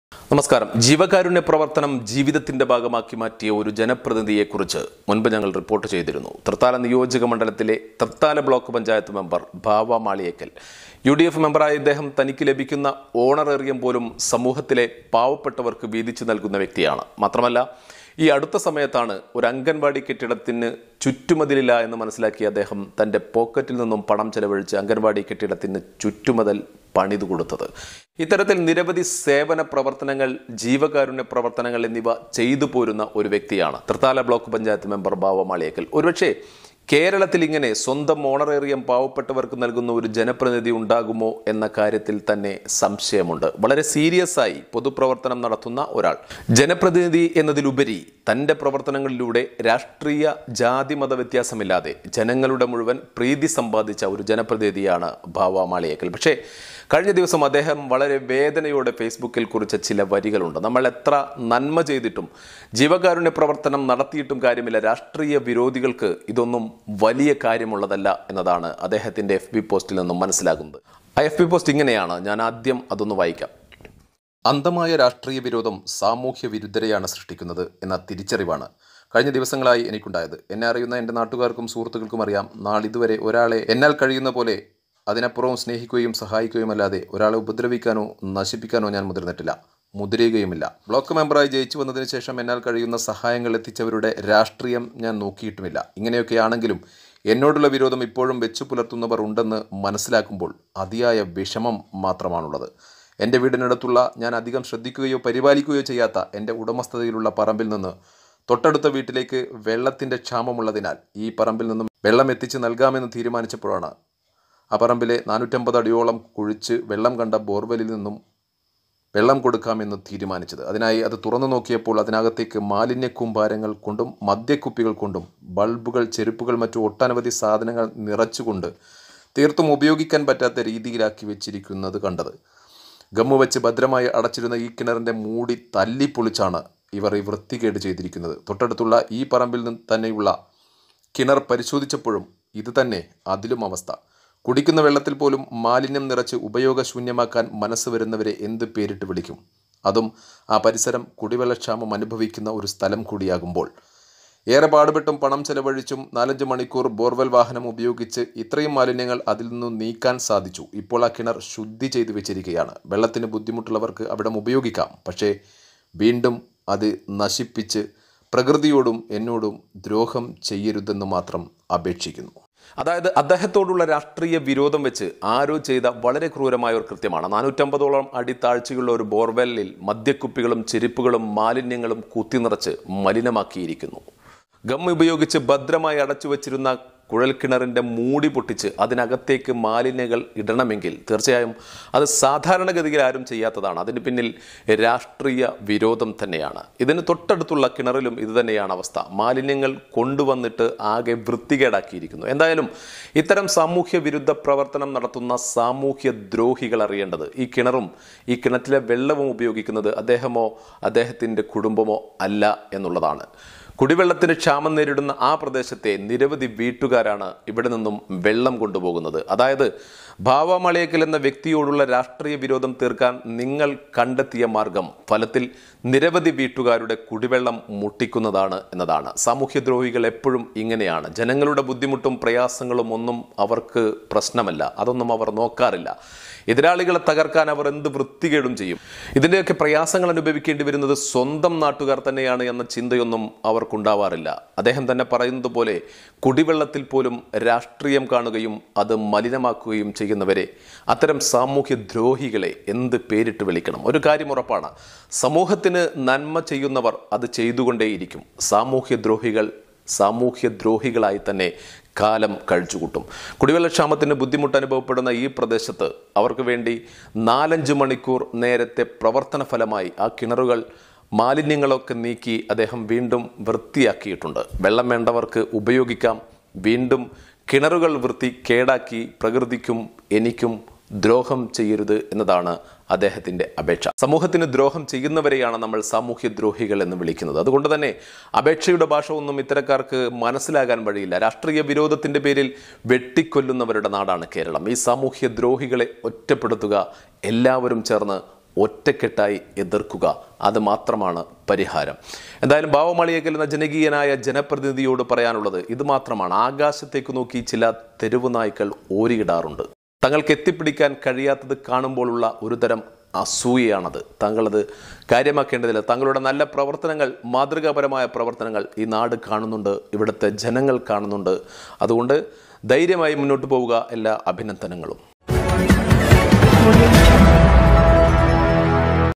രാഷ്ട്രീയ വിരോധത്തിൻ്റെ പേരിൽ കിണറുകൾ മലിനമാക്കി.
പരാതിയുമായി തൃത്താല ബ്ലോക് പഞ്ചായത്ത് മെമ്പർ